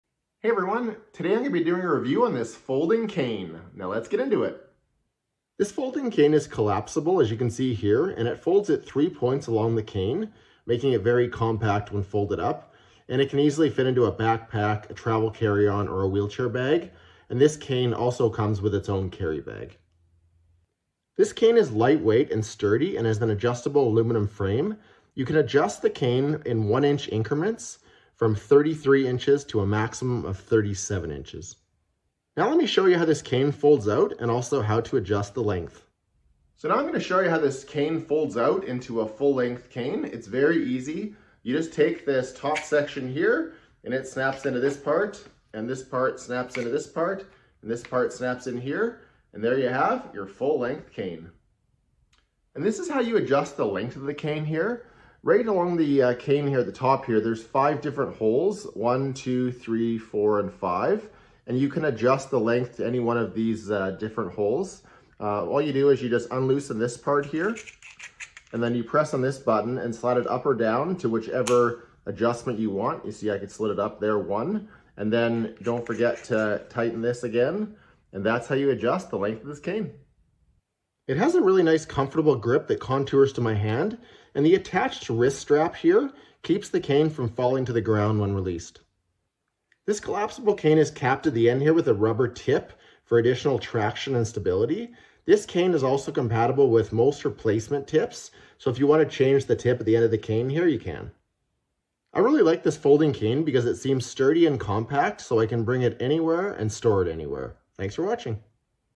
Vive Folding Cane Review & Demonstration
Check it out

The Vive folding cane is a great option for those who want extra stability while walking. Its collapsible design and lightweight and sturdy aluminum frame make it compact and easily transportable, fitting into a purse, backpack, carry-on, or wheelchair bag. Featuring an ergonomic hand grip and rubber tip, this cane also includes an integrated wrist strap.